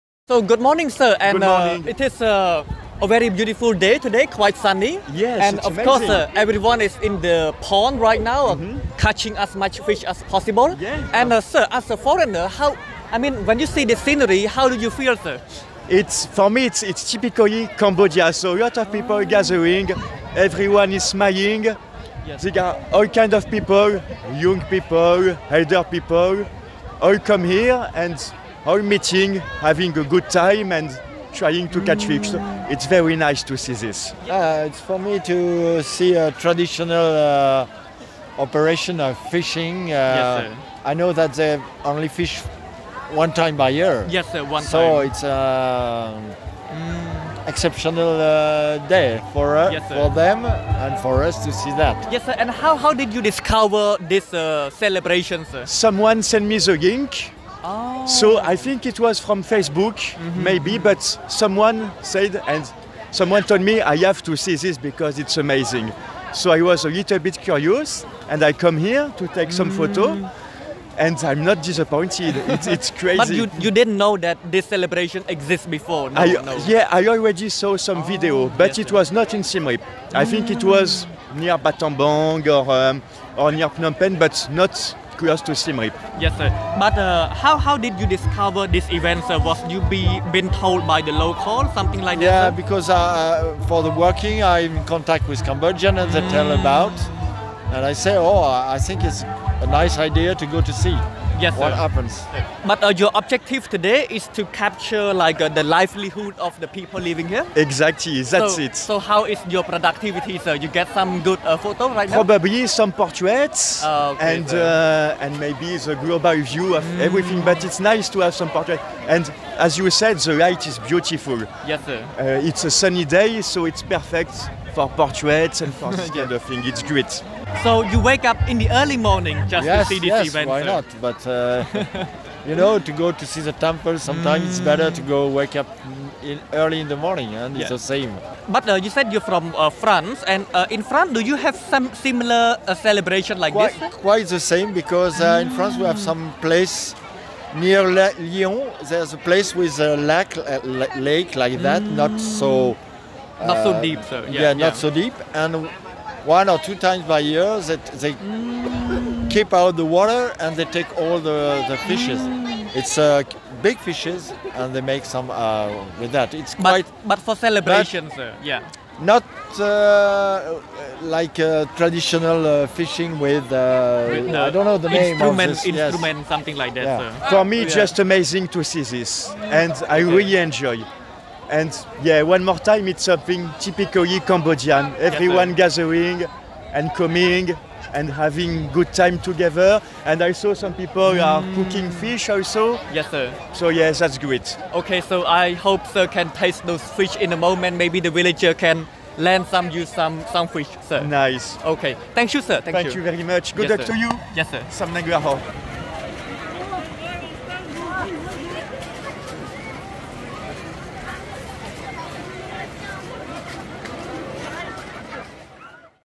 The Annual Fishing Celebration Attracts Foreign Visitors
SIEM REAP - Foreign visitors from France such as Remi Abad and Benoit Lafay spent their early morning visiting and observing the annual fishing celebration in the village of Bangkoang, Prasat Bakong district.

The celebration saw hundreds of local fishers race to catch as much fish as possible inside a large pond prior to the Lerng Meak celebration.

Held on Feb 11, the celebration marks the conclusion of the rice harvasting season. This tradition has passed down for generations.

This video was uploaded to Cambodianess Facebook page on February 12, 2024.